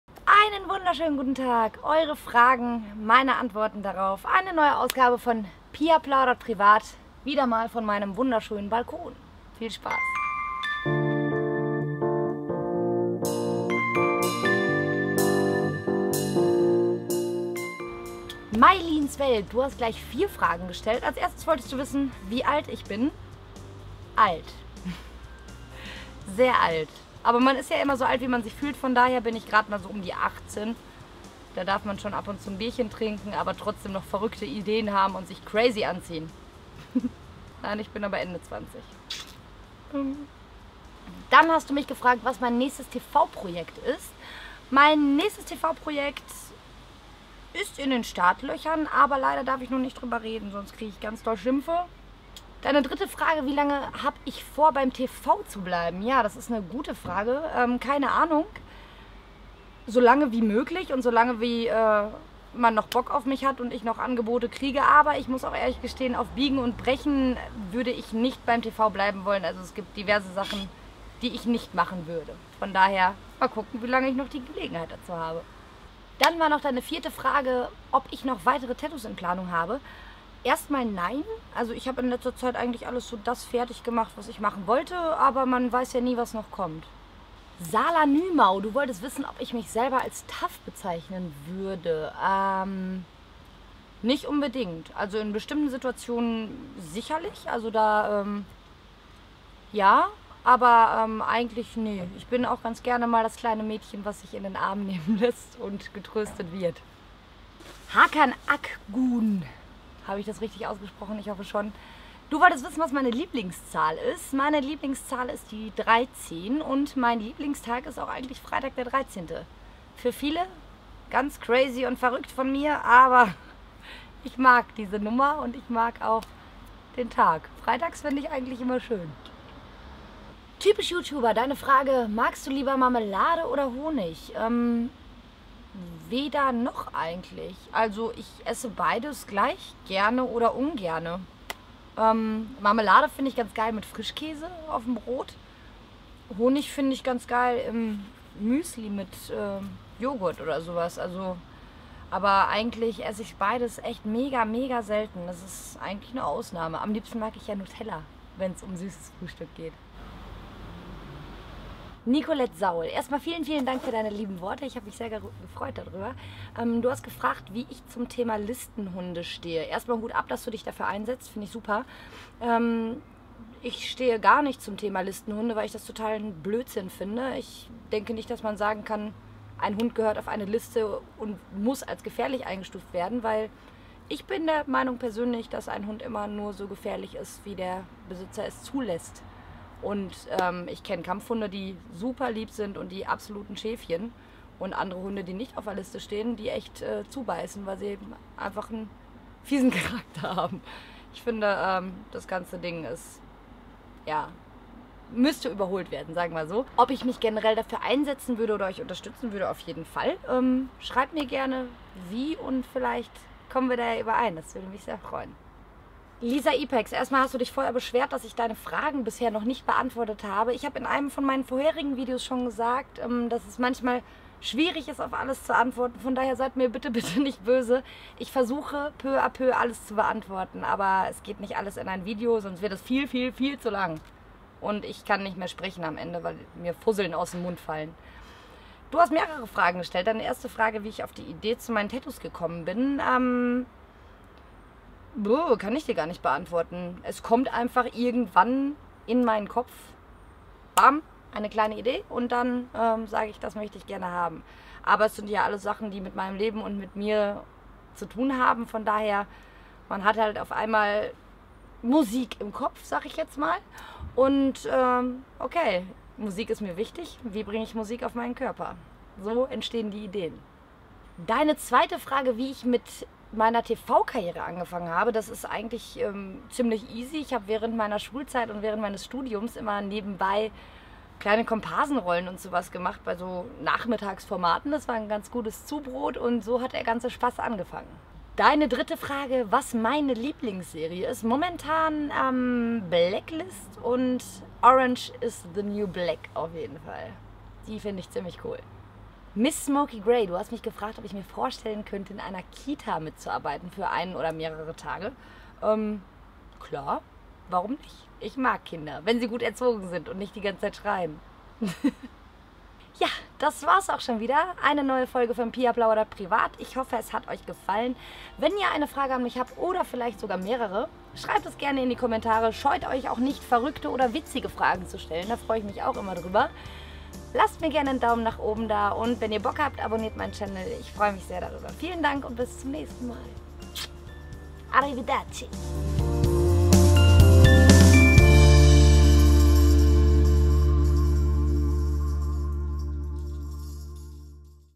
Pia plaudert privat #10
Es ist mal wieder Zeit für eine neue Plaudereinheit. Ich danke euch von Herzen für eure vielen Fragen und Kommentare. Bitte seit nicht traurig falls eure Frage nicht dabei war. Ich gebe mein Bestes möglicht viel zu beantworten, vielleicht ist Deine Frage ja heute mit dabei :